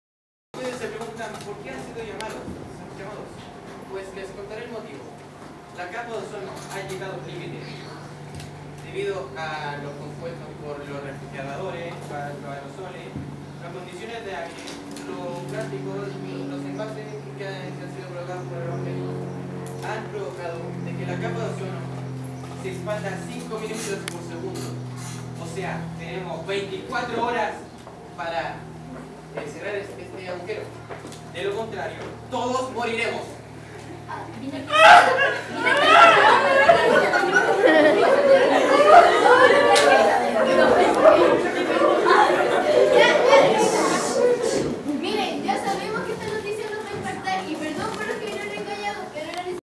Actuacion David Silva Teatro 2017
Actuación de David donde es un científico que con una inteligencia superior y apoyado a sus amigos superheroes intentan solucionar el problema de la capa de ozono